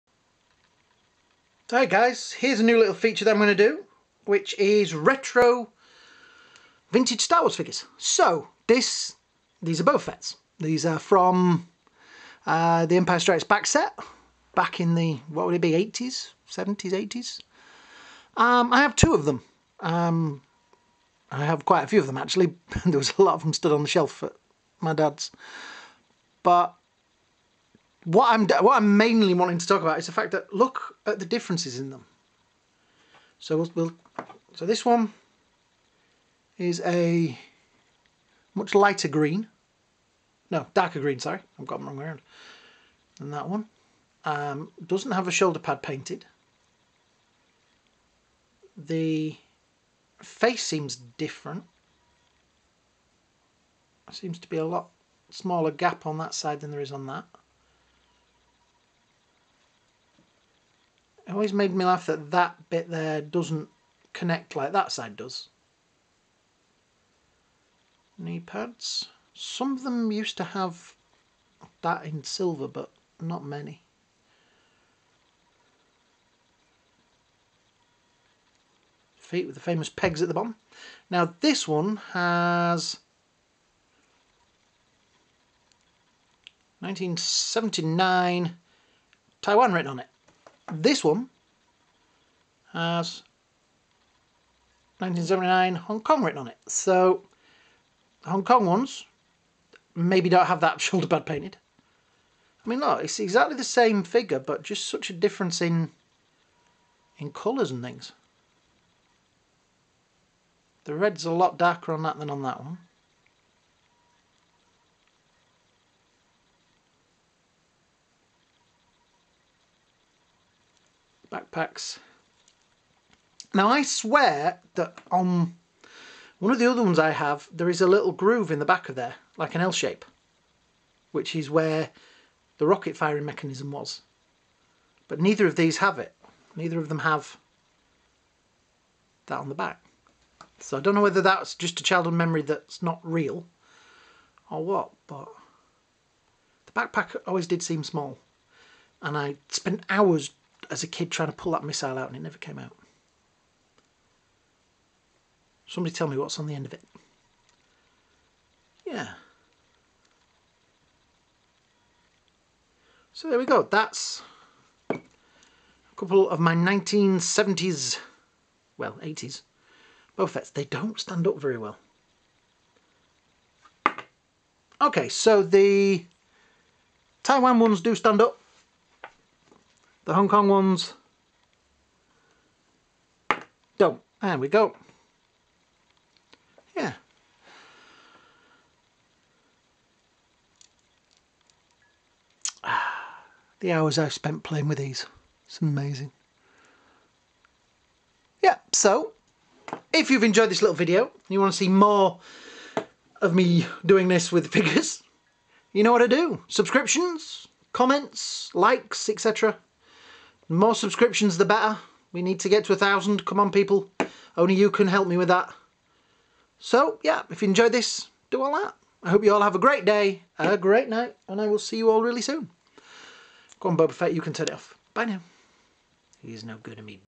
Vintage Boba Fett action figure comparison throwback review
I have a large collection of figures they are my favourite things !